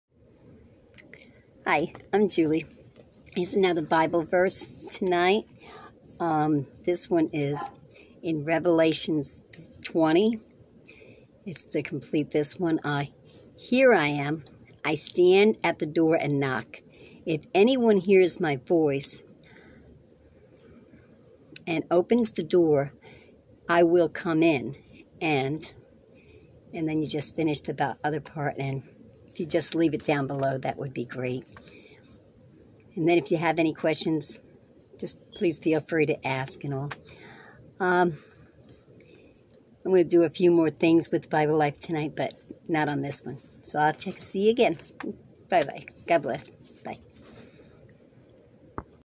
It's My Birthday 🎈🎉🎂 I Just Turned 60. Thank For watching March 21, 2021: Revelations 3:20
biblelife Thank you for making my birthday 🥳🎉🎈 special